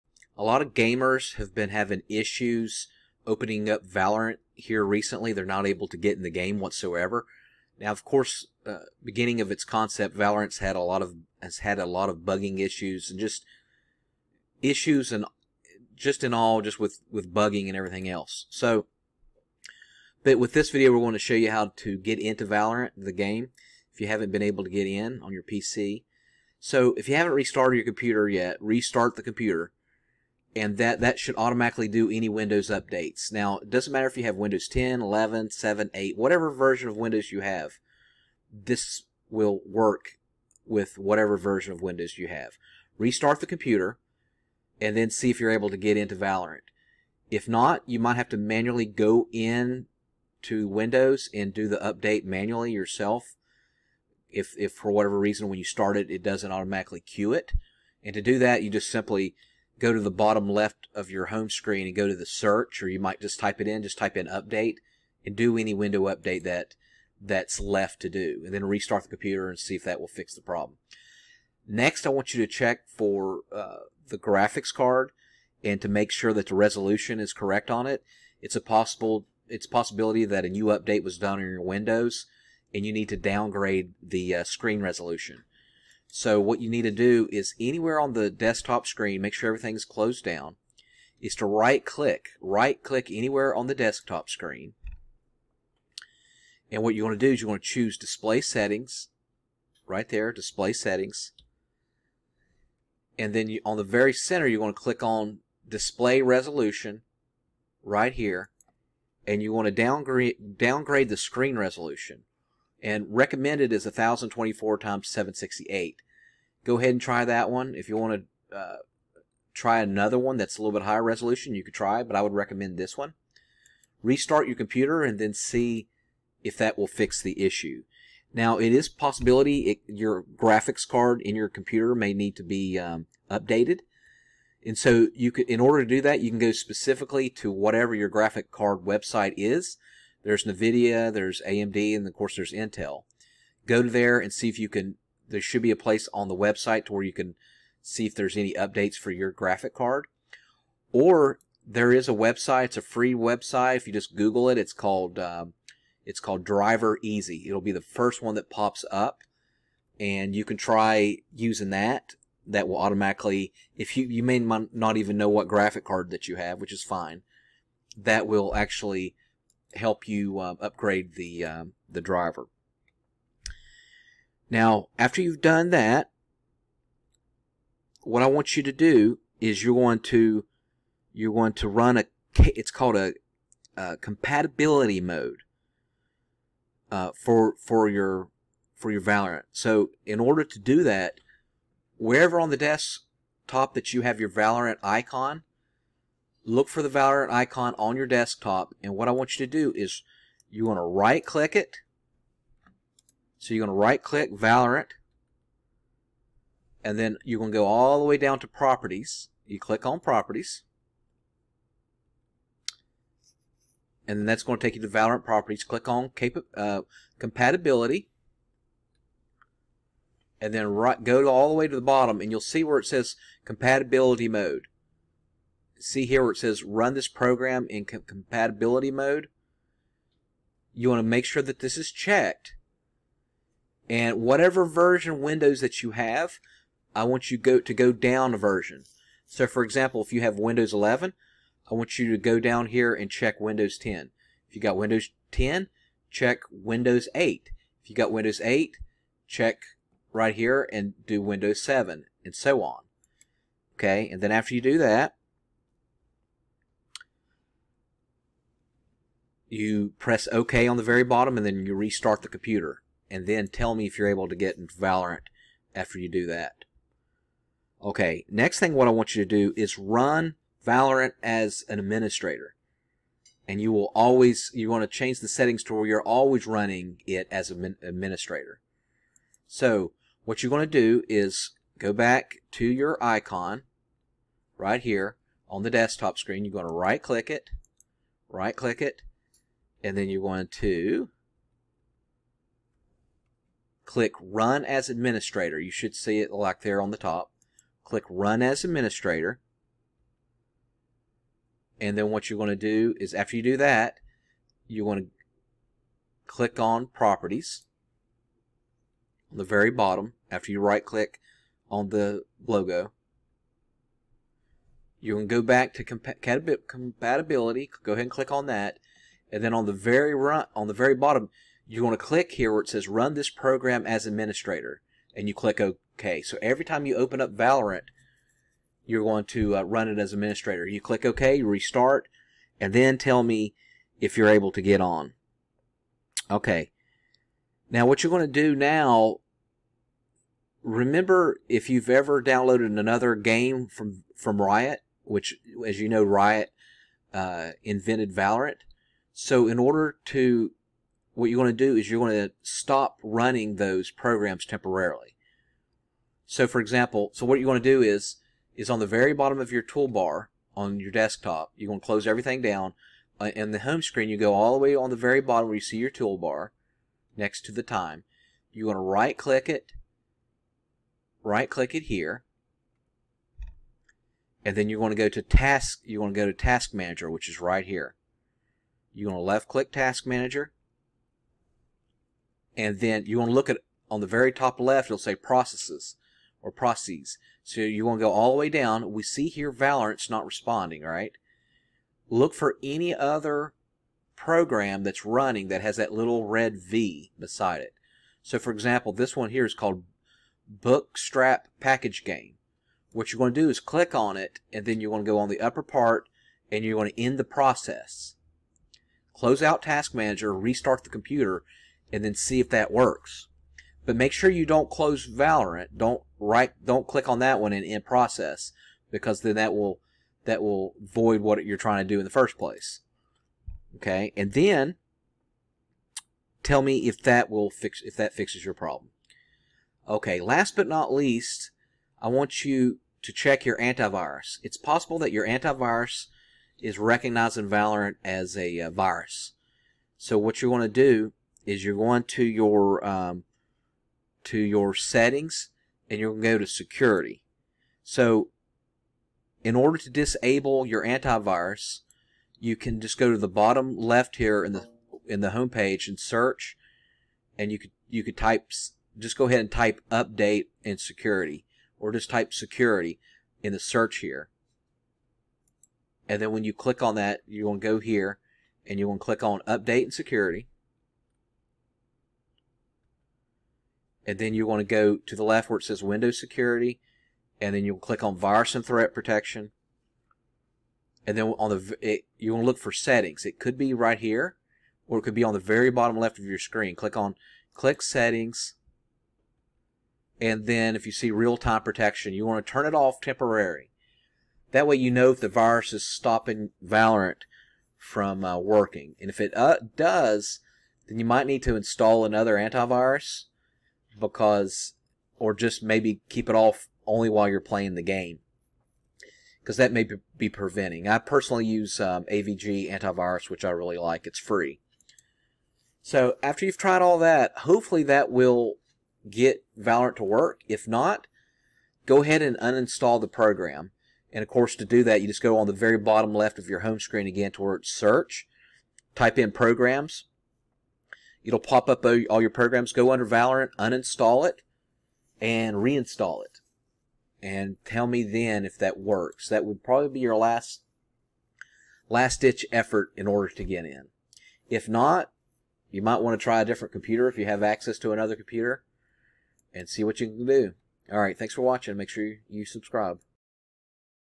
Fix VALORANT Not Opening Launching (Windows 10 11 7 Wont Open Up Work Launch Screen Click Icon)
This video will help you open up and launch the game Valorant on your PC laptop or desktop. A lot of people have been having issues getting Valorant to work. valorant Cant Open Valorant